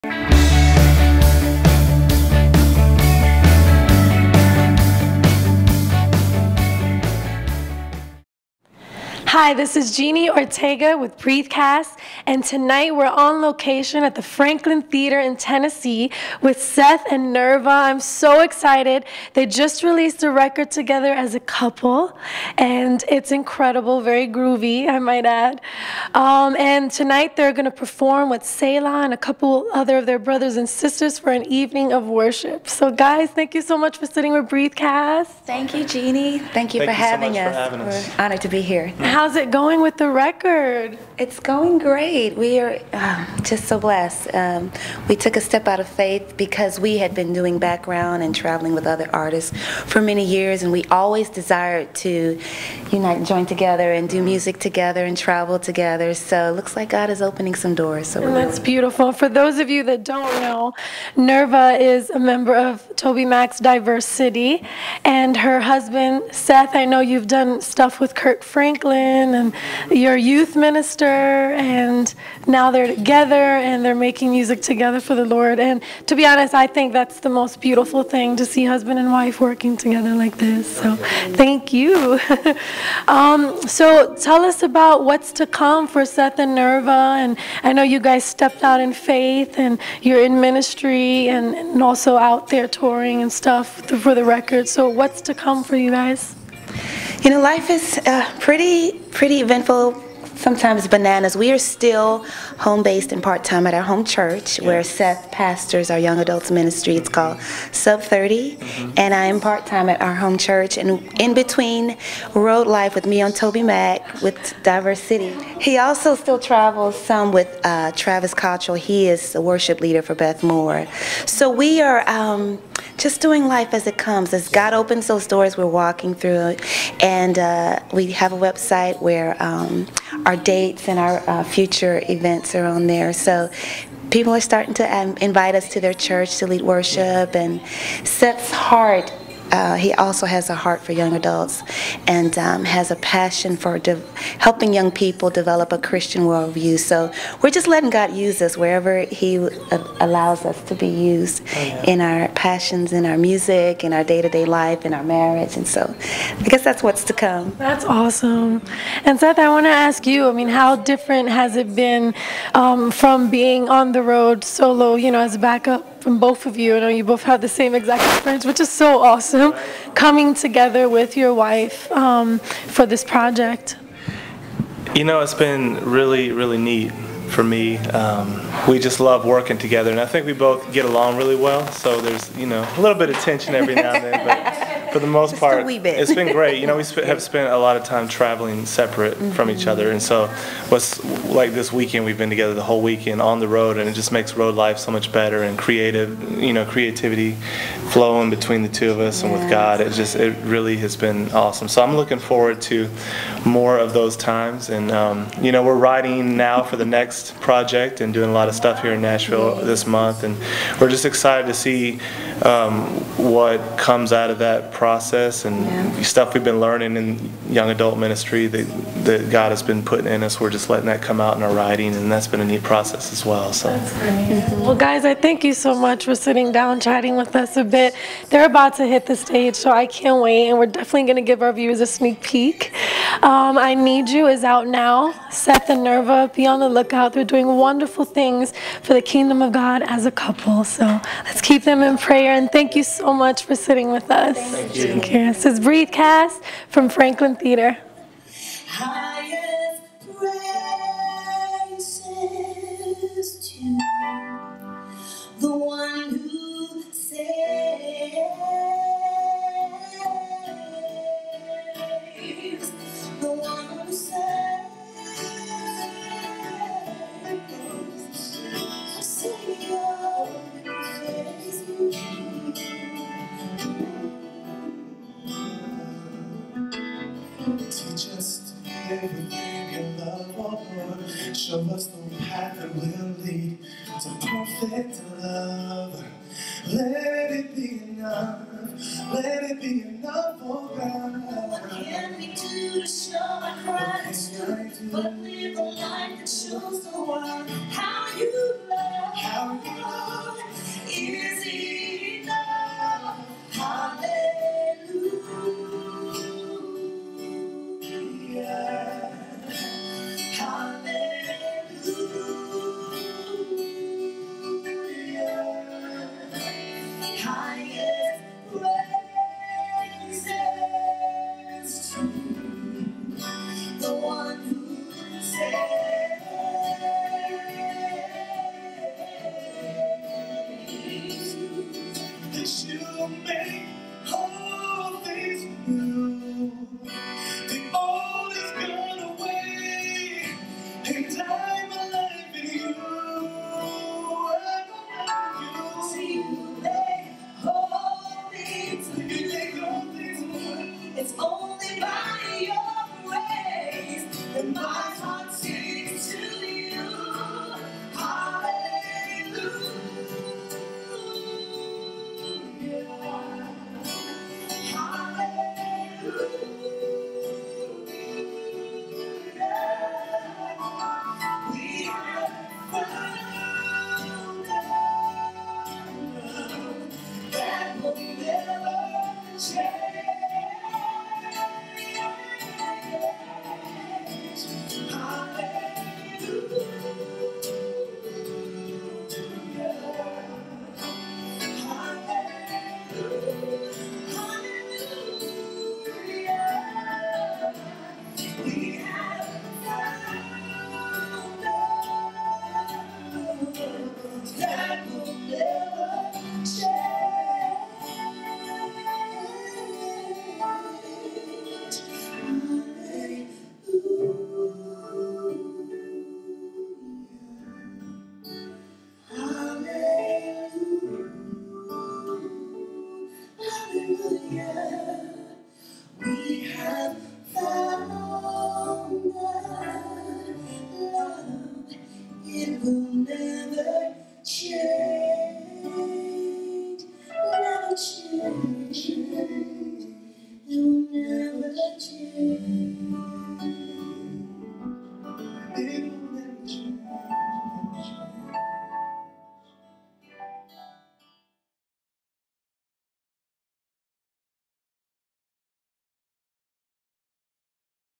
Seth & Nirva Talk Album/Perform for 'An Evening of Worship' at Franklin Theater | BREATHEcast.com
Check out BC Live from 'An Evening of Worship' in The Franklin Theater, TN with Christian Recording Artists - Seth & Nirva !!!

for the Latest Christian Entertainment News and so much more...